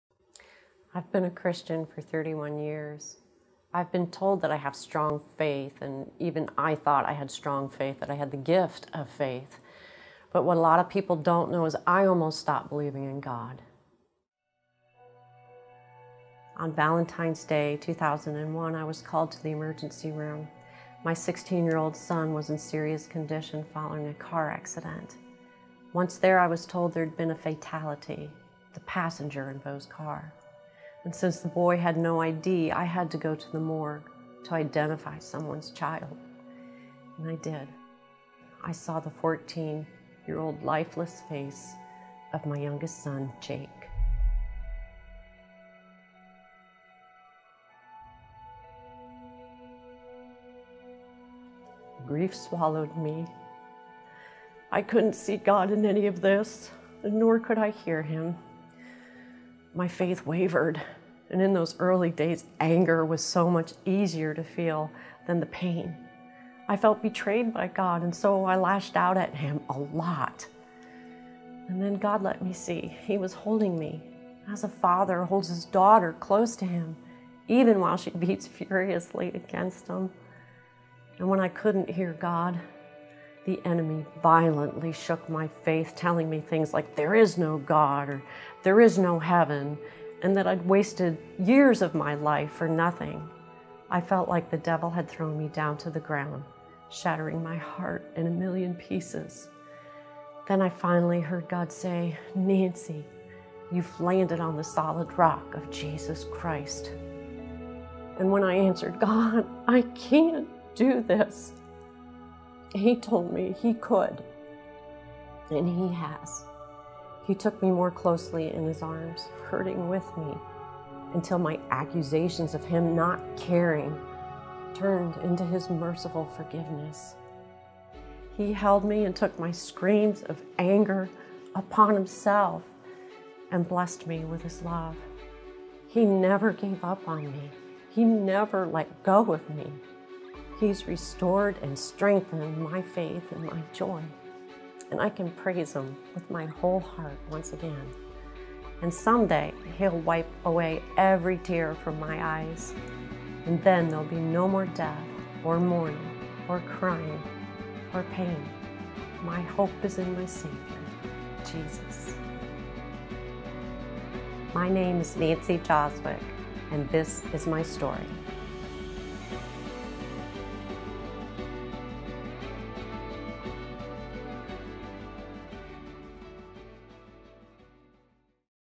Nancy Jozwiak myStory
On Valentines Day 2001, Nancy's faith was shaken and tested when a grave tragedy struck their family. Nancy wrestled with her relationship with Christ as she tried to see God in this situation, but the grief was to much to bare on her own. God took hold of Nancy and gave her peace as He walked her through this hardship. Today, Nancy's faith is stronger than ever. She loves to share this story in hopes that it will encourage others to draw near to God during their times of difficulty.